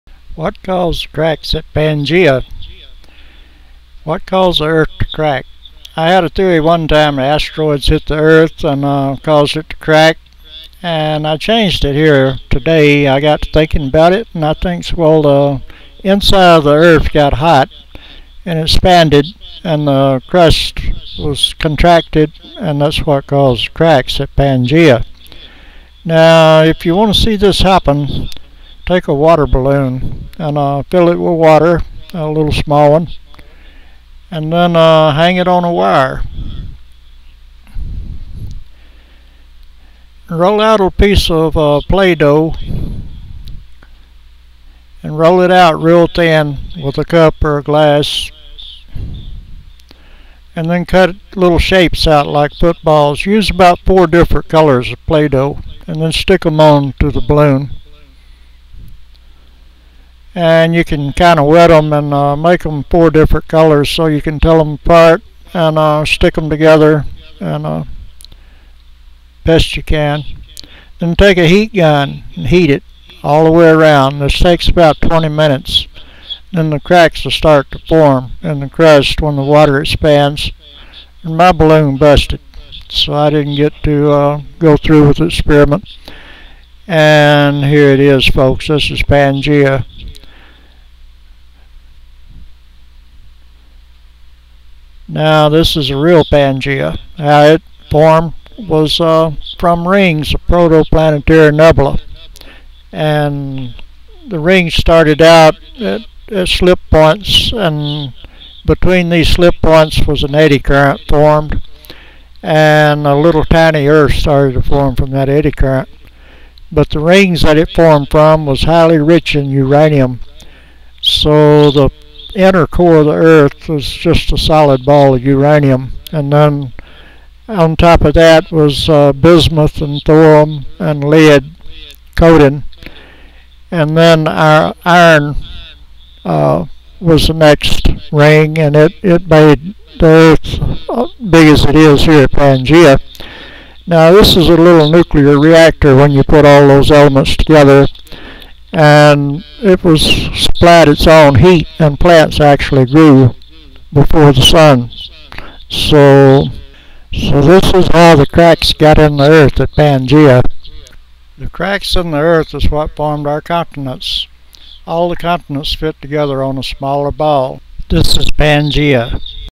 How the continents formed at Pangaea
This video shows how the continents split apart and the cause for that to happen.